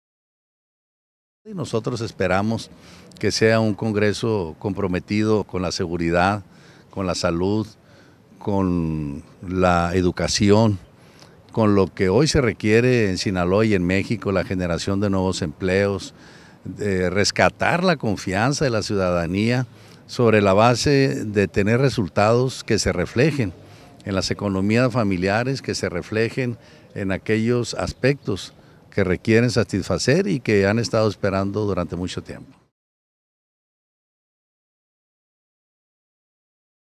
Rector de la U.A.S espera que los nuevos Diputados tengan compromiso en temas de educación
Espera el Rector de la UAS Juan Eulogio Guerra Liera que la 63 legislatura que este primero de octubre entró en funciones en el Congreso de Sinaloa tenga un compromiso con seguridad, con salud, con educación.

Guera Liera agradeció a los Diputados de la anterior Legislatura por el apoyo brindado a la máxima casa de estudios de Sinaloa.

Señaló el Rector de la U.A.S que espera en breve tener un acercamiento con los nuevos legisladores Sinaloenses.